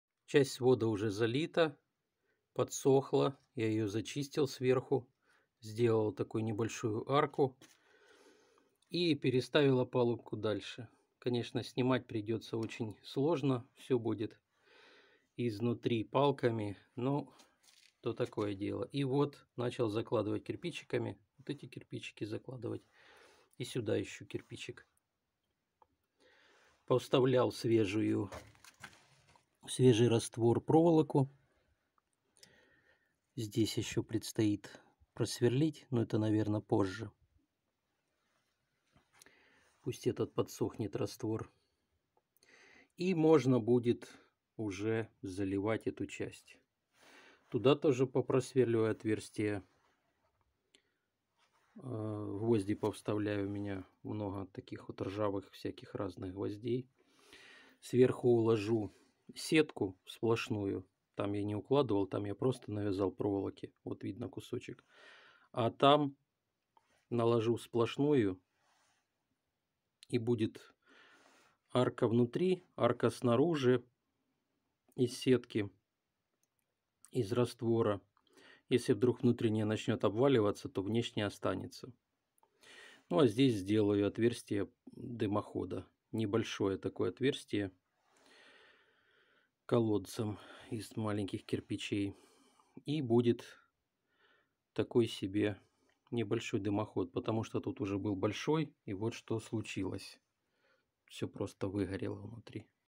Заливаю свод печи
Армирую и заливаю огнеупорным раствором собственной рецептуры свода топки печи.